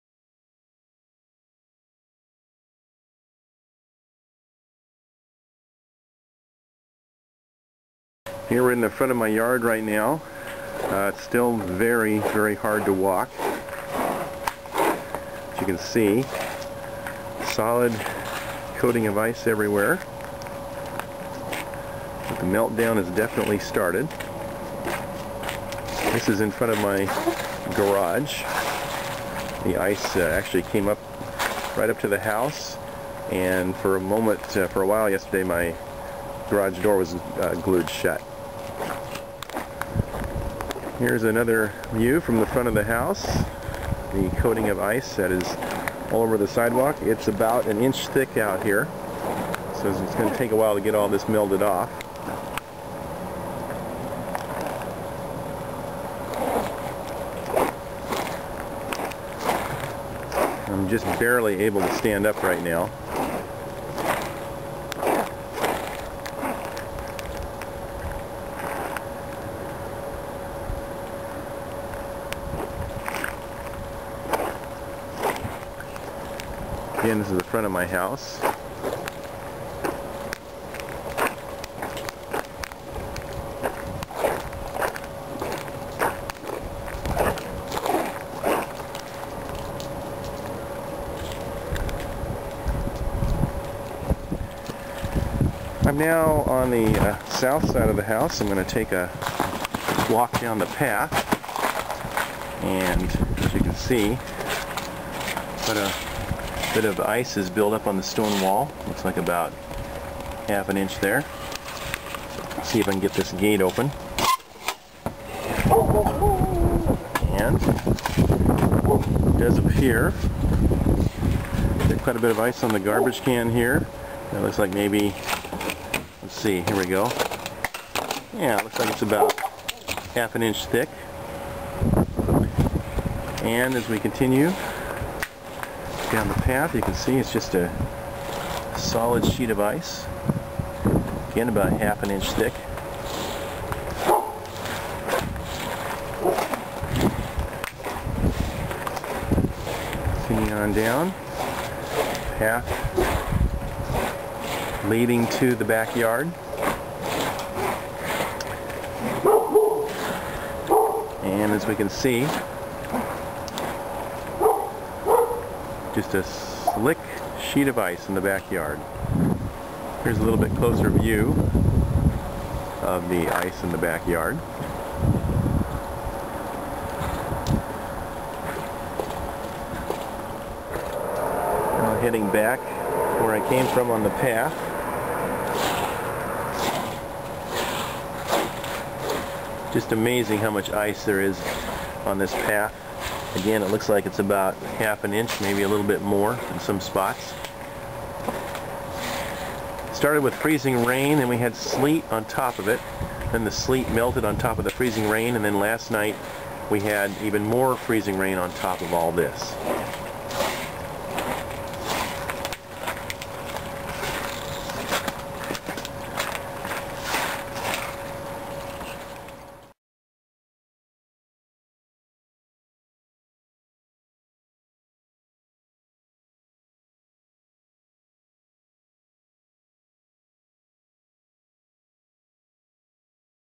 Hermiston Ice Storm Aftermath - Jan. 21, 2012
The Hermiston icing episodes began with freezing rain on Wednesday, Jan. 18, depositing a layer of ice about 1/4-inch thick.  This was followed by about 1/4-inch of sleet that melted into the first layer of ice on Thursday, Jan. 19.  By the end of Friday afternoon, more freezing rain followed on top of the ice and sleet, making the street slick enough for ice skating.  When this video was made at 9:30 AM Saturday morning, temperatures had finally risen into the upper 30s.

This was by no means an "epic" ice storm, but it was one of the more serious storms of the past decade and affected much of the Pacific Northwest.  Local schools were closed for three days, and Interstate 84 experienced a lengthy closure.